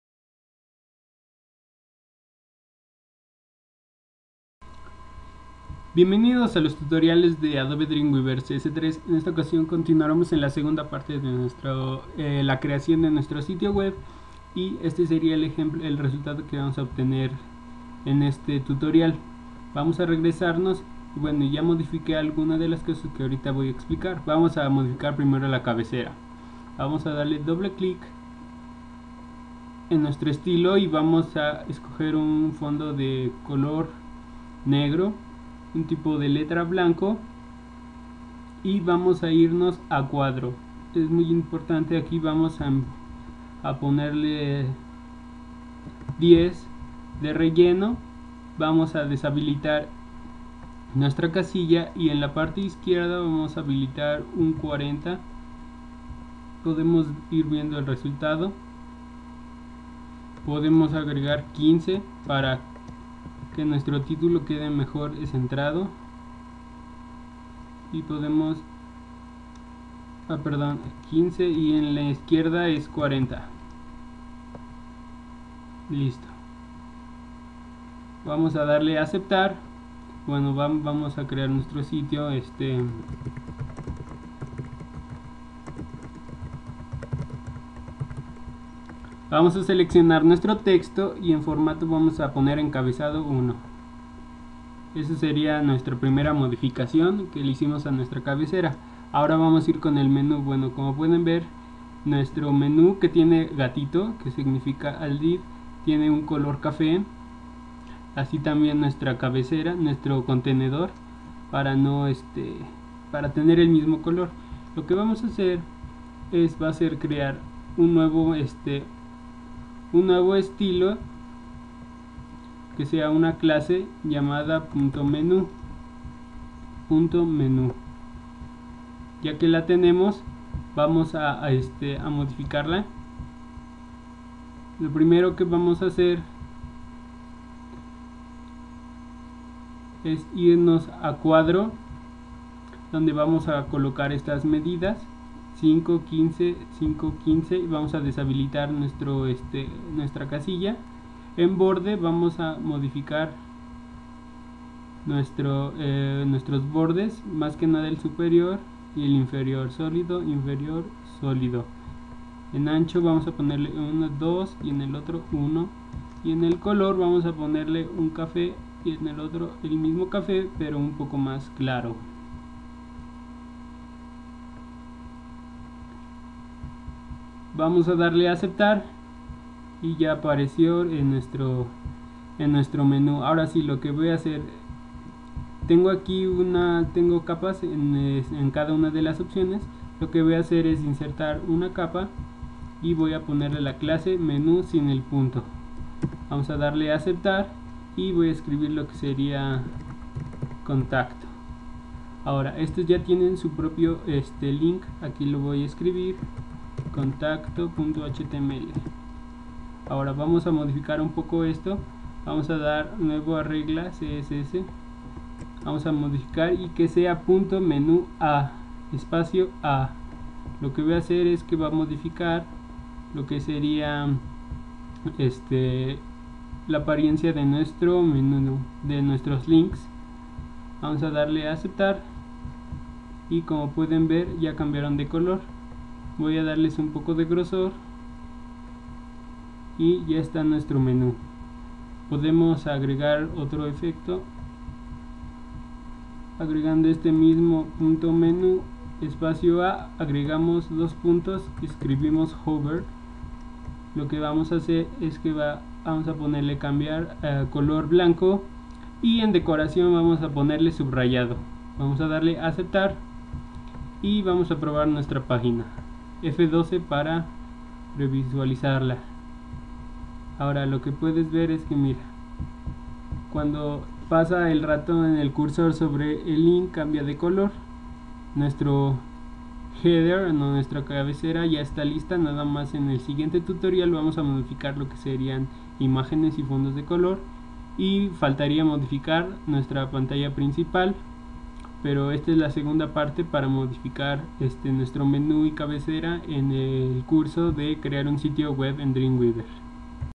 Adobe Dreamweaver CS3 | Crear un sitio web parte 2
Continuamos con nuestro curso para hacer un sitio web, en este tutorial voy a enseñar a aplicar más hojas de estilo para diseñar el menú y la cabecera de nuestro sitio